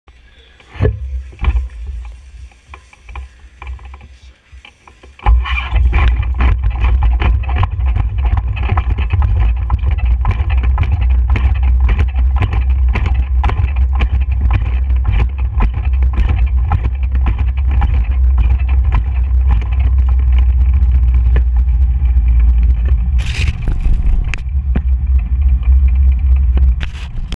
Ben Brown Roller Sprint 2 23/01/16
Hit 40.6km/hr Focused on starts, hand speed, power and form